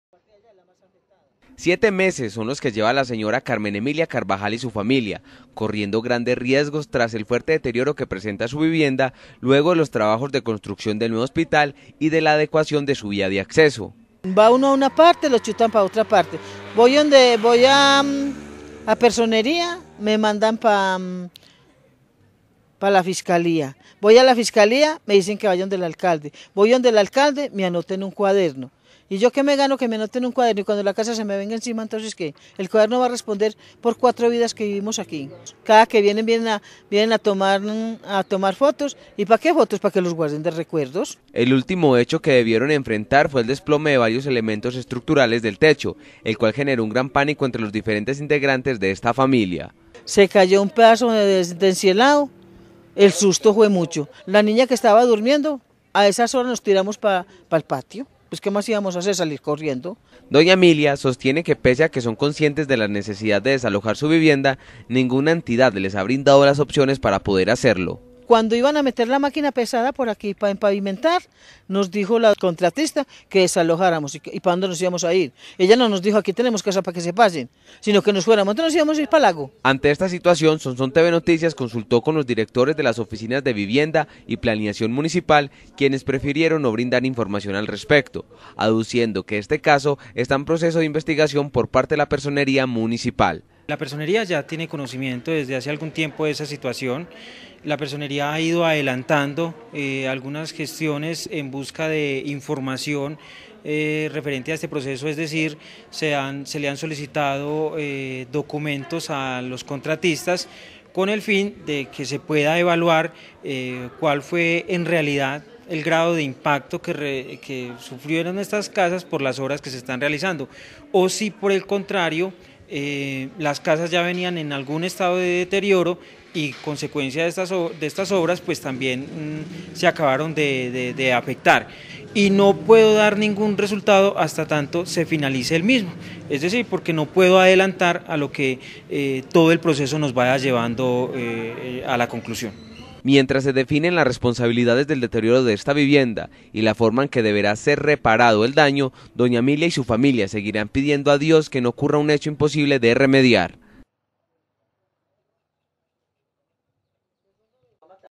Siete meses lleva sin intervención vivienda afectada por obras del nuevo hospital
Siete meses son los que lleva la  señora Carmen Emilia Carvajal y su familia  corriendo grandes riesgos tras el fuerte deterioro que presenta su vivienda luego de los trabajos de construcción del nuevo hospital y de la adecuación de su vía de acceso.